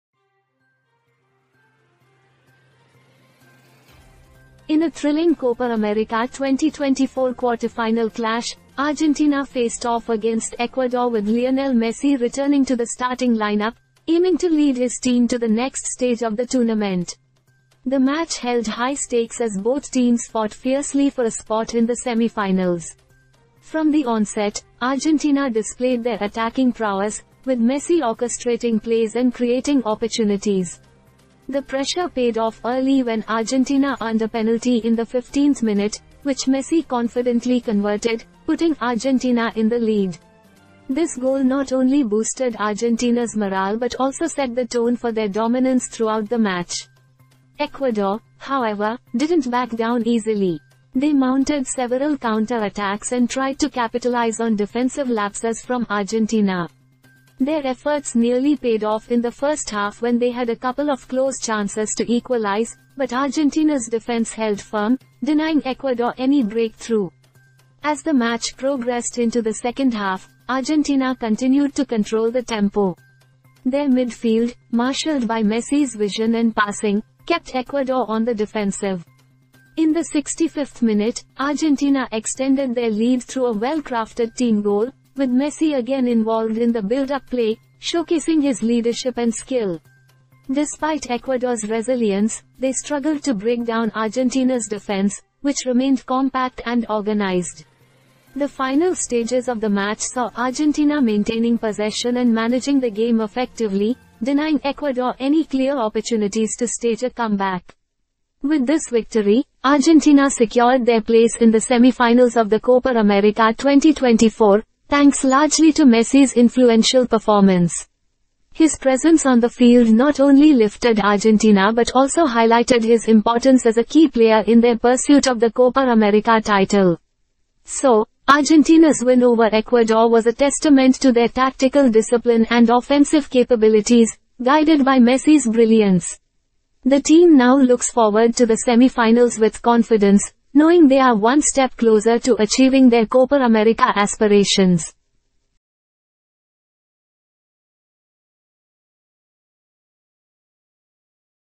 Messi Leads Argentina to Victory Against Ecuador in Copa America Quarterfinal
00:10 Messi Leads Argentina to Victory Against Ecuador in Copa America Quarterfinal

Messi, Argentina, Copa America, Quarterfinal, Victory, Ecuador, Football, Soccer, GOAT, Winning, News,Hot News,Sports,Sports News,Fitness